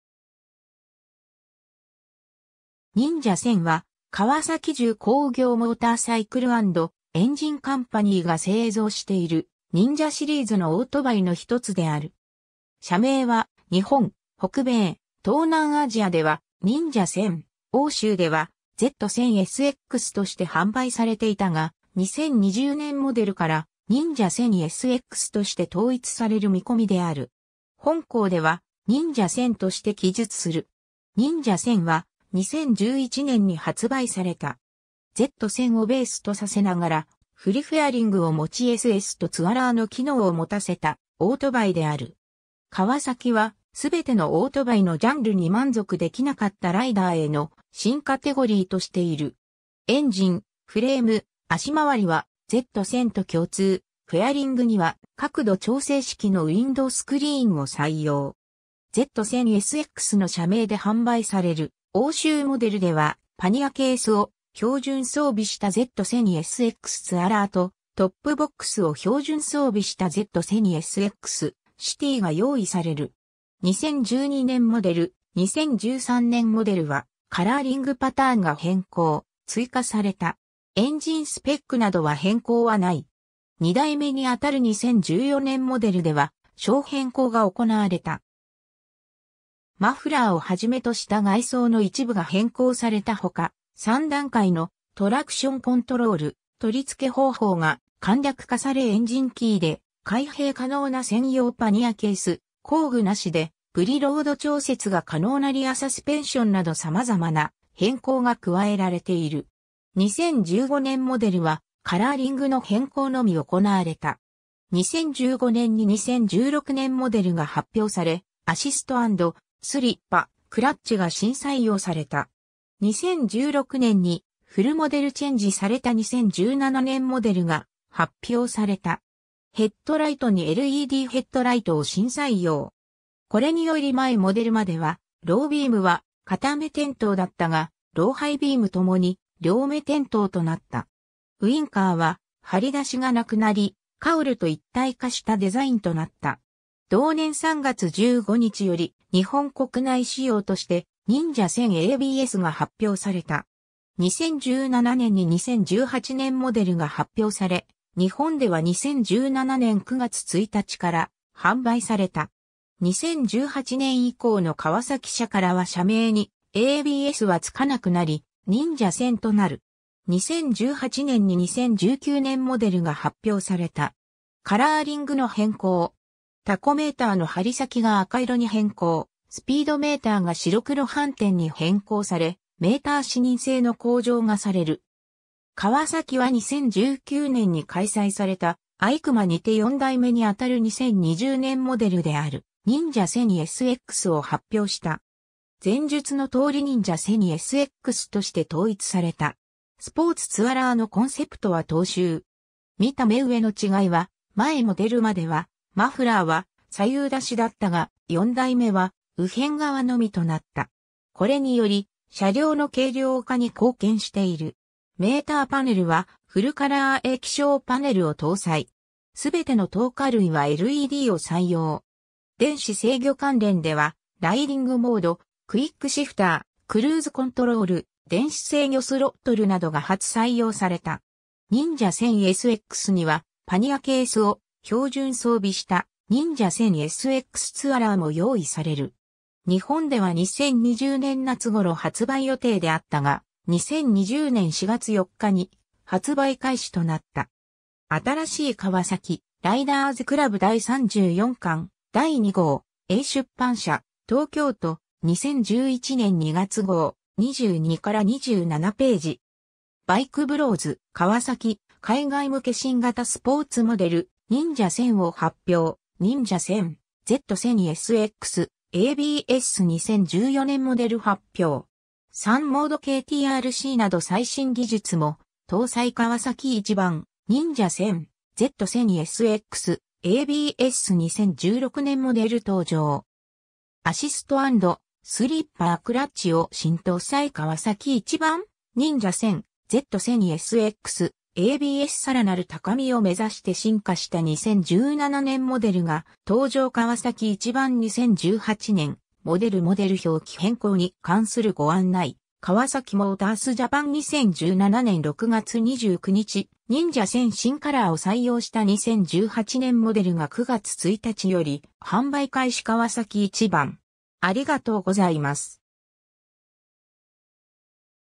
カワサキ・ニンジャ1000
ニンジャ1000（Ninja1000）は川崎重工業モーターサイクル&エンジンカンパニーが製造しているニンジャシリーズのオートバイの一つである。
車名は日本・北米・東南アジアではNinja1000、欧州ではZ1000SX（ゼット1000エスエックス）として販売されていたが、2020年モデルからNinja1000SXとして統一される見込みである。
本項ではニンジャ1000として記述する。
 ニンジャ1000は、2011年に発売された。
Z1000をベースとさせながらフルフェアリングを持ちSSとツアラーの機能を持たせたオートバイである。
カワサキは、全てのオートバイのジャンルに満足できなかったライダーへの新カテゴリーとしている。
 エンジン、フレーム、足回りはZ1000と共通、フェアリングには角度調整式のウインドウスクリーンを採用。
Z1000SXの車名で販売される欧州モデルでは、パニアケースを標準装備したZ1000SX Tourerと、トップボックスを標準装備したZ1000SX Cityが用意される。
 2012年モデル、2013年モデルはカラーリングパターンが変更・追加された。
エンジンスペックなどは変更はない。
 2代目にあたる2014年モデルでは小変更が行われた。
マフラーをはじめとした外装の一部が変更された他、3段階のトラクションコントロール、取付方法が簡略化されエンジンキーで開閉可能な専用パニアケース、工具なしでプリロード調節が可能なリアサスペンションなど様々な変更が加えられている。
 2015年モデルはカラーリングの変更のみ行われた。
 2015年に2016年モデルが発表され、アシスト&スリッパ―クラッチが新採用された。
 2016年に、フルモデルチェンジされた2017年モデルが発表された。
ヘッドライトにLEDヘッドライトを新採用。
これにより前モデルまではロービームは片目点灯だったが、ロー・ハイビームともに両目点灯となった。
ウインカーは張り出しがなくなり、カウルと一体化したデザインとなった。
同年3月15日より日本国内仕様として Ninja 1000 ABS が発表された。
 2017年に2018年モデルが発表され、日本では2017年9月1日から販売された。
2018年以降のカワサキ車からは車名にABSは付かなくなりNinja 1000となる。
 2018年に2019年モデルが発表された。
カラーリングの変更。
タコメーターの針先が赤色に変更、スピードメーターが白黒反転に変更されメーター視認性の向上がされる。
（カタログには掲載されてないが販売店などにはメーカーから通知が写真付きで来ている。） カワサキは2019年に開催されたEICMAにて4代目にあたる2020年モデルであるニンジャ1000SXを発表した。
前述のとおりニンジャ1000SXとして統一された。
スポーツ+ツアラーのコンセプトは踏襲。
見た目上の違いは、前モデルまではマフラーは左右出しだったが、4代目は右片側のみとなった。
これにより車両の軽量化に貢献している。
メーターパネルはフルカラー液晶パネルを搭載。
すべての灯火類はLEDを採用。
電子制御関連では、ライディングモード、クイックシフター、クルーズコントロール、電子制御スロットルなどが初採用された。
ニンジャ1000SXにはパニアケースを標準装備したニンジャ1000SXツアラーも用意される。
日本では2020年夏頃発売予定であったが、2020年4月4日に発売開始となった。
 ^ 「新しいカワサキ」『RIDERS CLUB』第34巻第2号、エイ出版社、東京都、2011年2月号（2010年12月27日発行）、 22-27頁。
 ^ BikeBros.「カワサキ、海外向け新型スポーツモデル「Ninja1000」を発表」 ^ ［Ninja 1000/Z1000 SX/ABS］2014年モデル発表! 3モードKTRCなど最新技術も搭載川崎壱番 ^ ［Ninja 1000/Z1000SX/ABS］2016年モデル登場。
アシスト&スリッパークラッチを新搭載川崎壱番 ^ ［Ninja 1000/Z1000SX/ABS］さらなる高みを目指して進化した2017年モデルが登場川崎壱番 ^ 2018年モデル モデル表記変更に関するご案内 - カワサキモータースジャパン・2017年6月29日（ABS装備が義務化されるため） ^ ［Ninja 1000］新カラーを採用した2018年モデルが9...